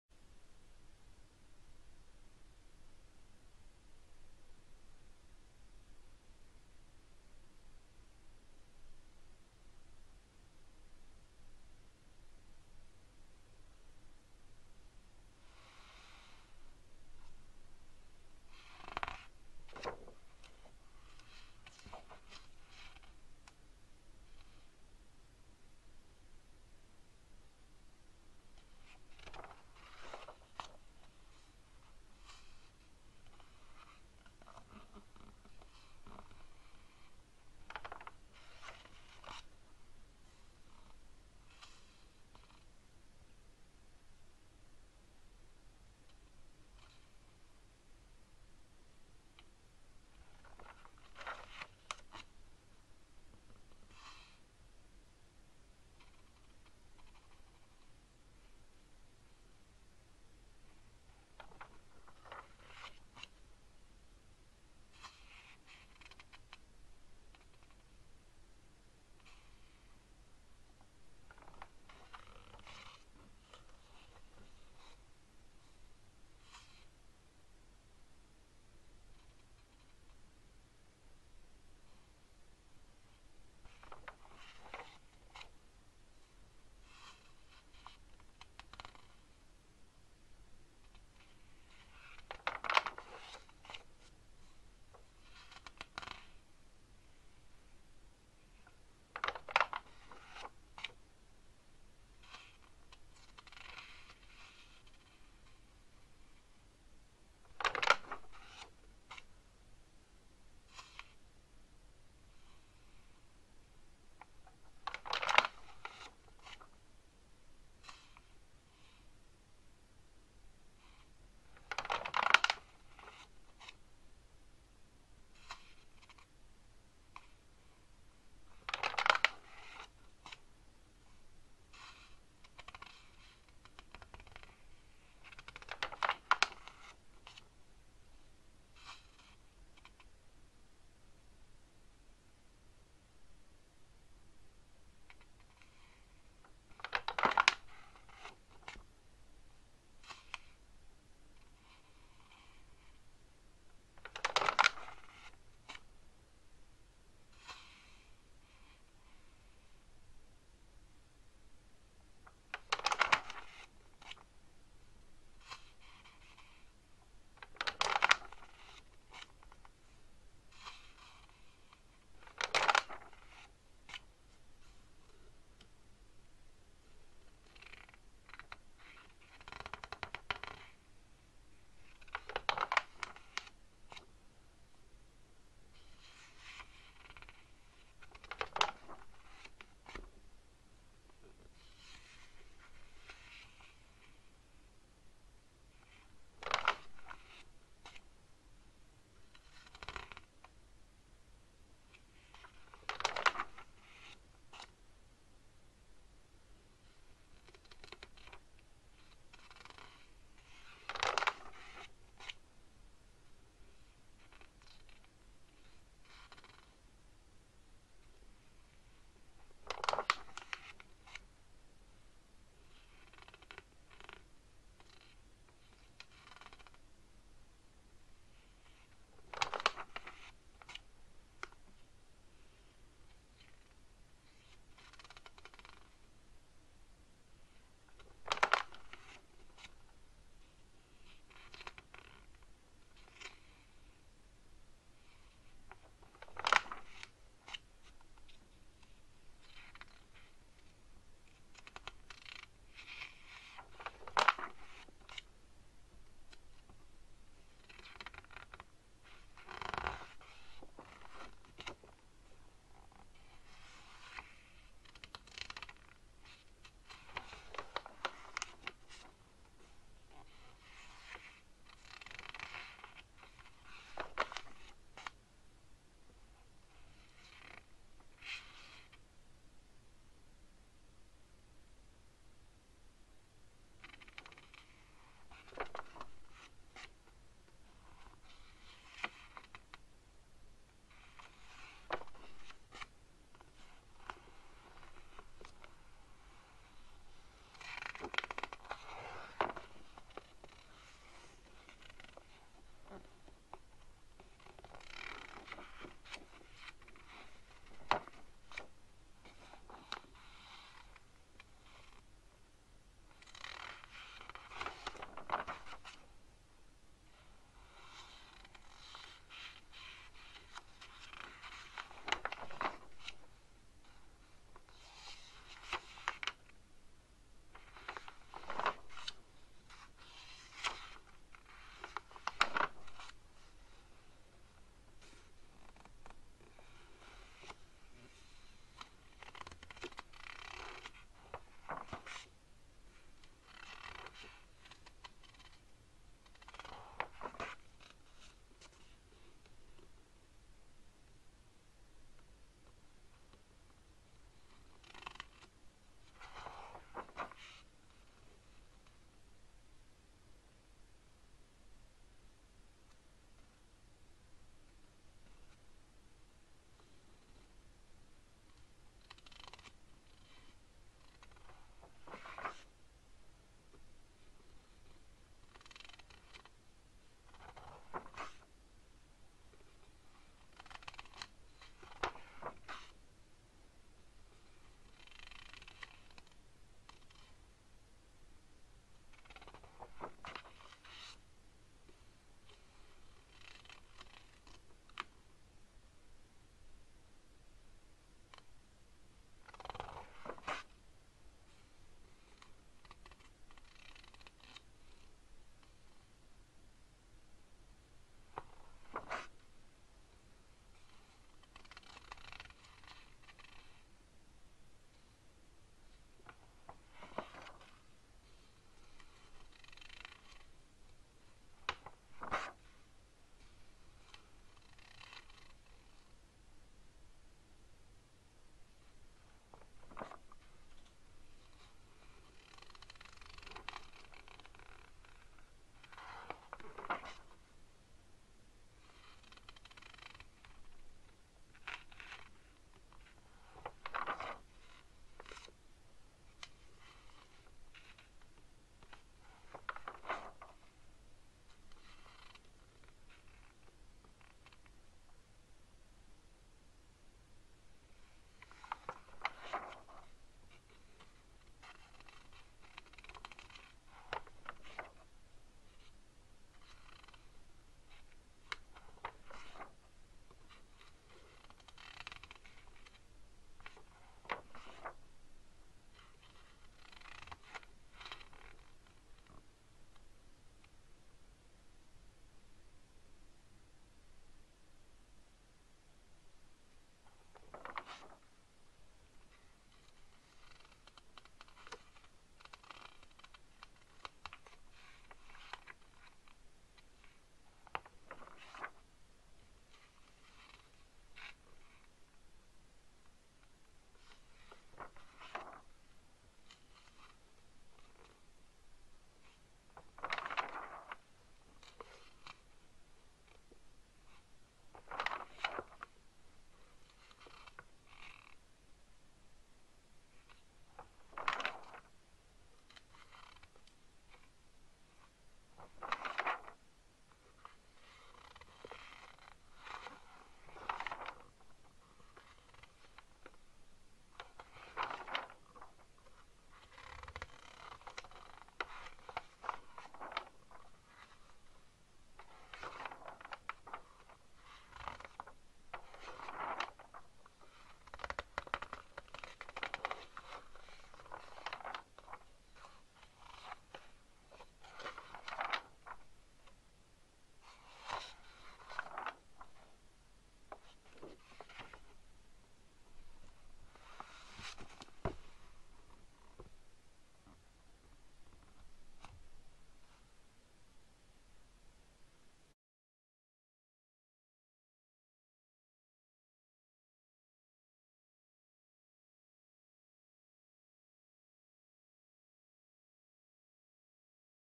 1975 April ASMR Magazine Flip Through: Harper's Bazaar Pam Suthern, Beverly Johnson
Vintage issue of Harper's Bazaar April 1975.  Relaxing ASMR sounds from page turning and a step back in time at the same time.

Pam Suthern on the cover. A complete guide to your health.  The secrets of looking better, feeling healthier, younger and sexier.  April shimmers so you may flower.  Lisa Palmer's timeliest make-up tricks. Beverly Johnson's brightest color secrets. The sunshine summer of fresh flower prints. The flower accessory. Flower-print dresses for the city life.  The flower lingerie.